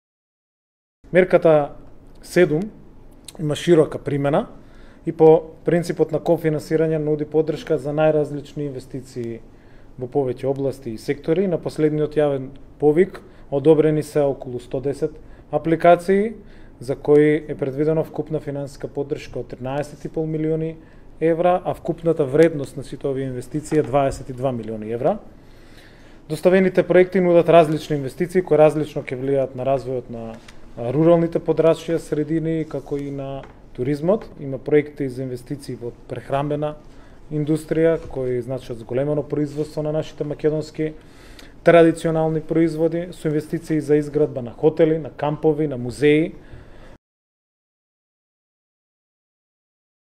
Николовски: Одобрени се 110 апликации од ИПАРД 2 за развој на мали бизниси
Министерството за земјоделство одобри 110 договори на апликанти за финансиска поддршка од мерката ИПАРД 2 за развој на мали бизниси.